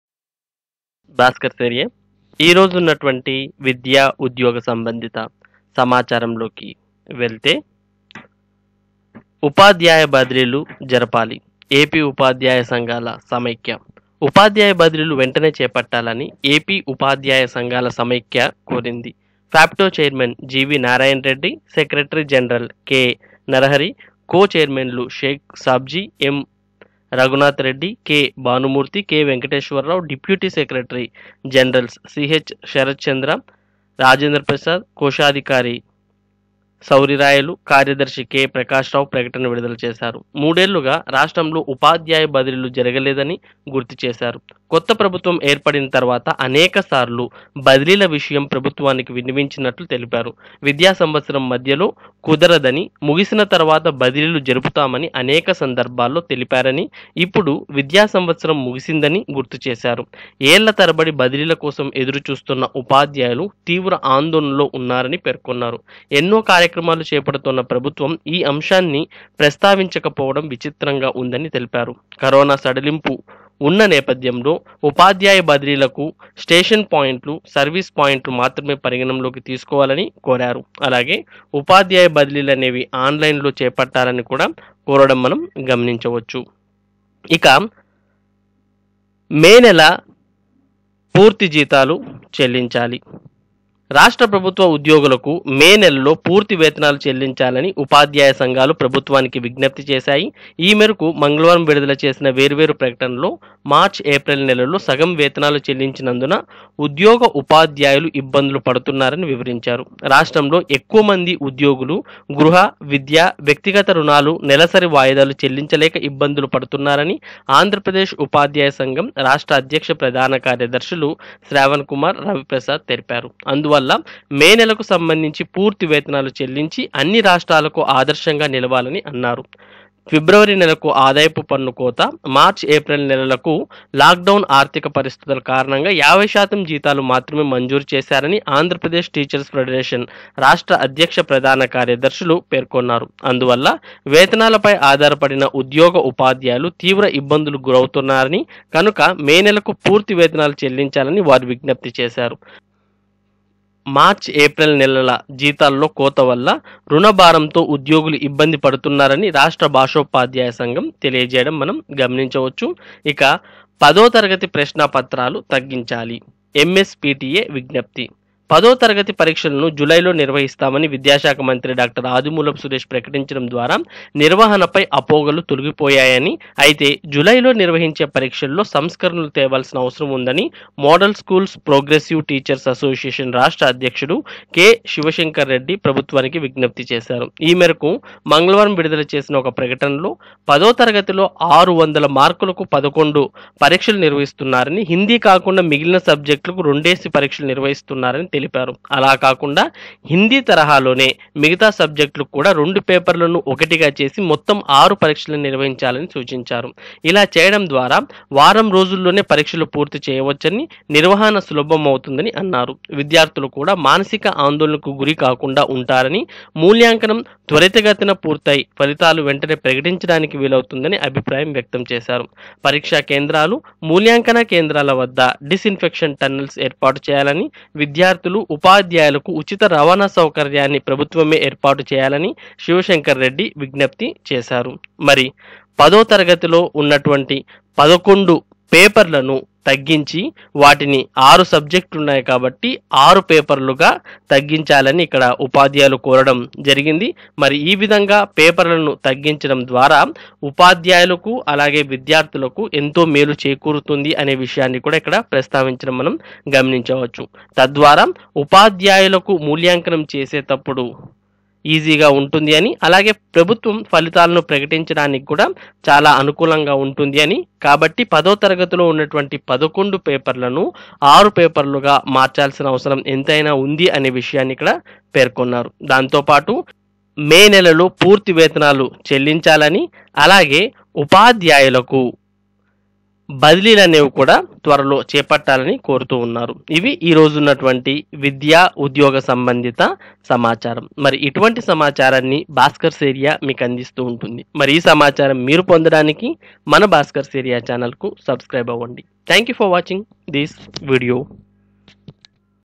ఉపాధ్యాయ బదిలీలు చేపట్టాలి| మే నెల పూర్తివేతనాలు చెల్లించాలి|10 తరగతి ప్రశ్నాపత్రాలను 6కు తగ్గించాలి
ఉపాధ్యాయ బదిలీలు వెంటనే చేపట్టాలి| మే నెల పూర్తి వేతనాలు చెల్లించాలి| పదో తరగతి ప్రశ్నా పత్రాలను 6కు తగ్గించాలి